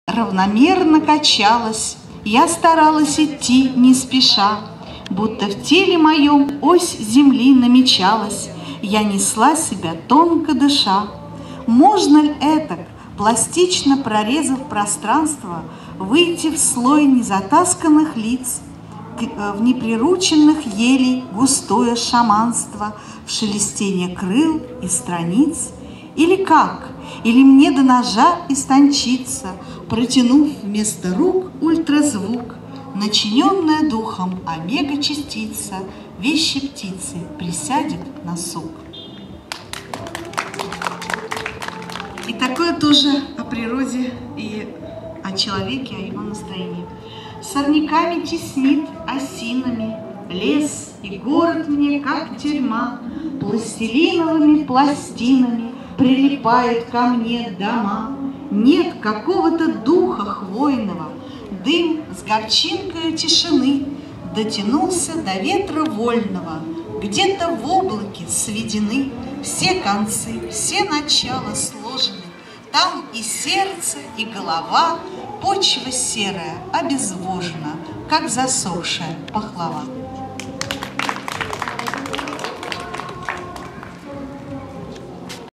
Зульфия Алькаева читает авторские стихи в санатории Саки.
19 августа 2017 г.
3-ий Международный литературно-музыкальный конкурс-фестиваль "Интеллигентные сезоны".
Выездной концерт в санаторий Саки.